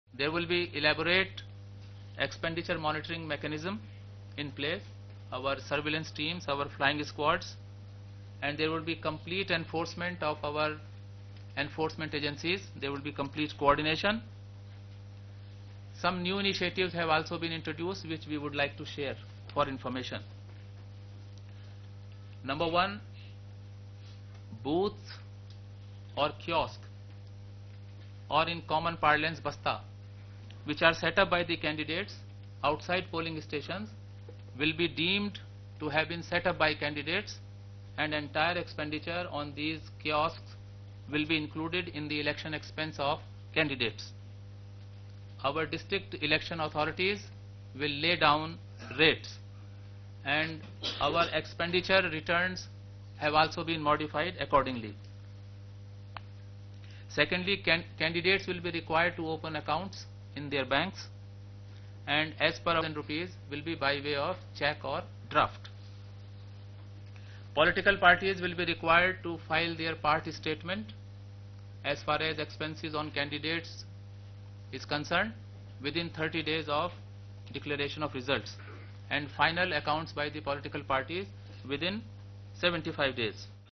CEC | Elaborate expenditure monitoring mechanism to be in place
Chief Election Commissioner Nasim Zaidi said there will be elaborate expenditure monitoring mechanism in place and there will be complete enforcement of their enforcement agencies. Some new initiatives have also been taken by the Election Commission which is as follows: Booth or kiosks, which are set up by the candidates outside the polling stations, will be deemed under the candidate’s own expenses. District election authorities will lay down rates and expenditure returns have been modified recently. Candidates will be required to open accounts in their banks. Political parties will be required to file their part statements. Candidates will be required to file their expenses within 30 days of the result and the political parties have to file their expenses within 75 days of the result.